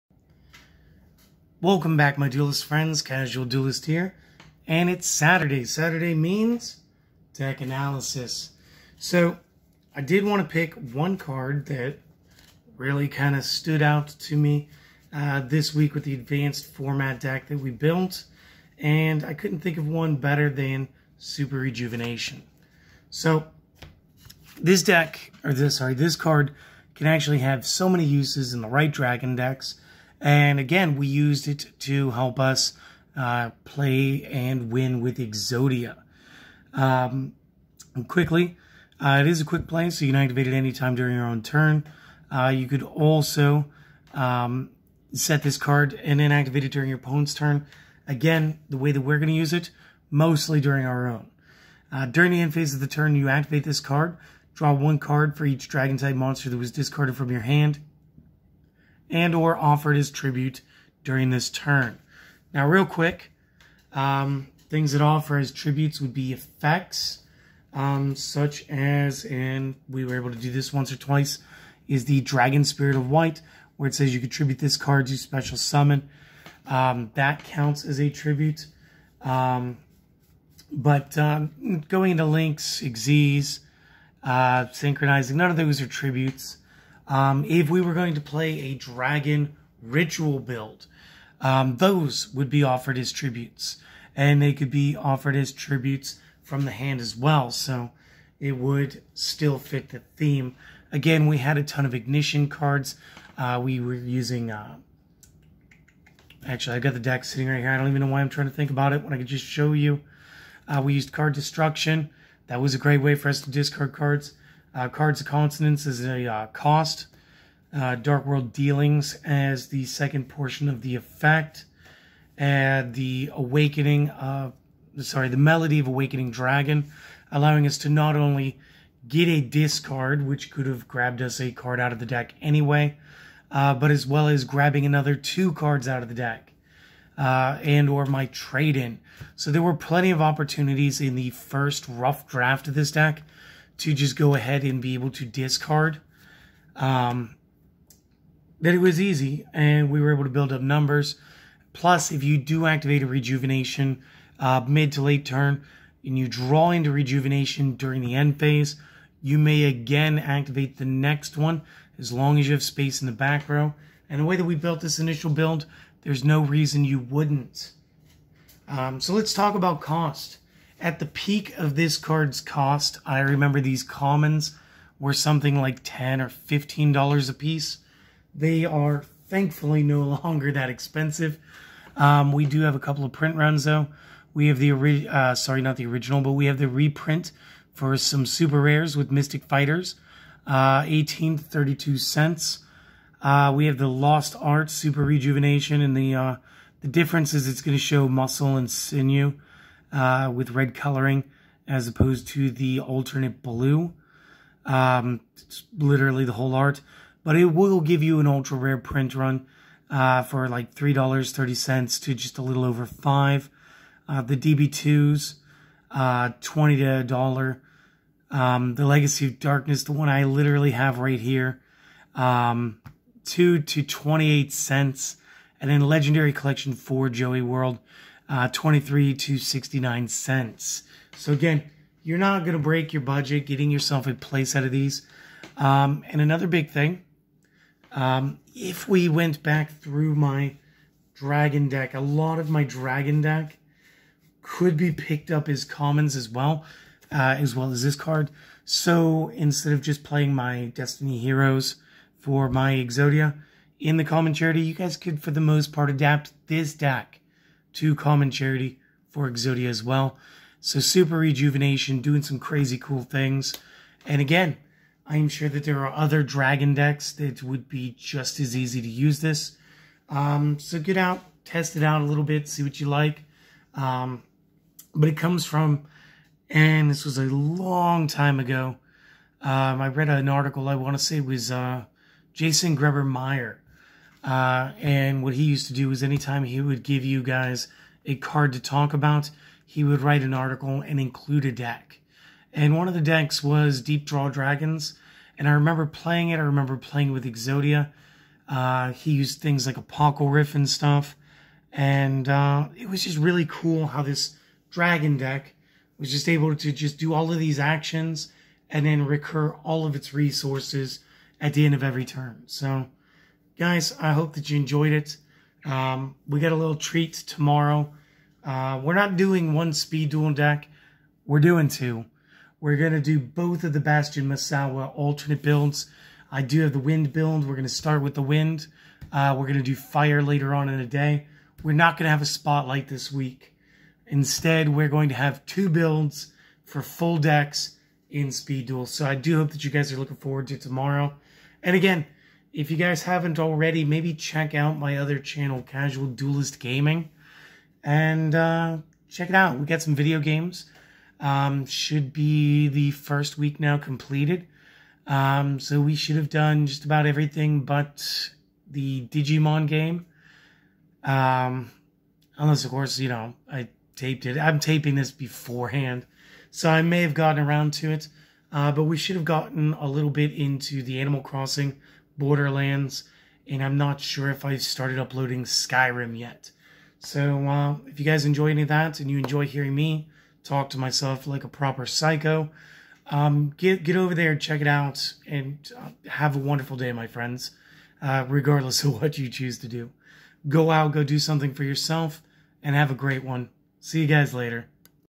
Tech Analysis: Super Rejuvenation
This weeks tech analysis will feature the main card from the deep draw dragon engine, Super Rejuvenation.  This card is thankfully budget again, but at one point was worth a minimum of $10 a copy.  I do hope you all enjoy this, and of course, I hope you all have a great day.